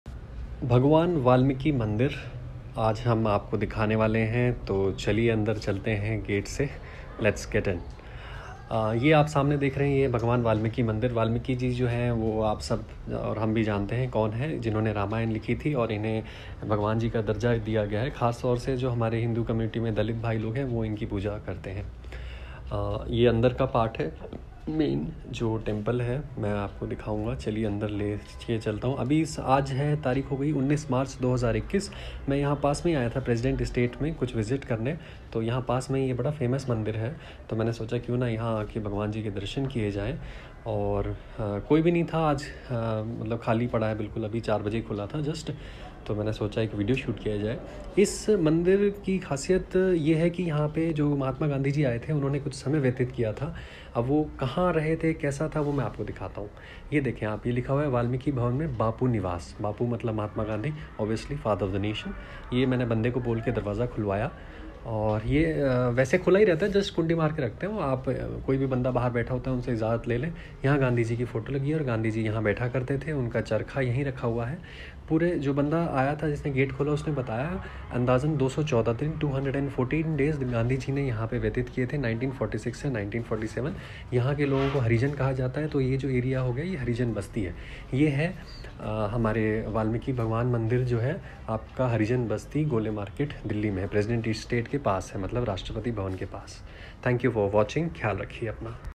Bhagwan Valmiki Mandir , Delhi
This is Bhagwan Valmiki Mandir in Harijan Basti in Delhi. It was visited by Indian PM Modi in the past and it has a special Bapu Nivas in it where Gandhiji stayed here for 214 days. I visited and prayed in the temple and also saw the Bapu Nivas.